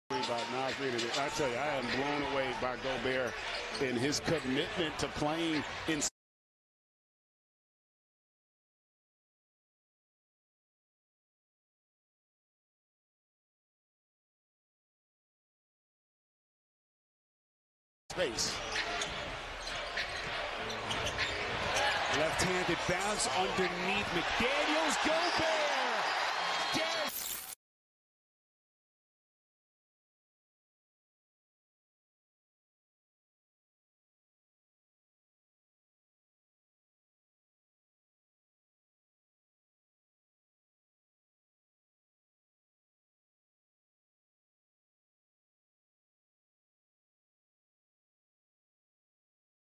bowser2bowser I really like the creative ways Chris Finch has used Gobert on offense including the t
bowser2bowser I really like the creative ways Chris Finch has used Gobert on offense including the two asymmetrical alignments 5 Slot amp 45 Out that seem be growing around the league In 45 Out empty WS corner Gobert sets a seemingly decoy flare screen and then rolls for a dunk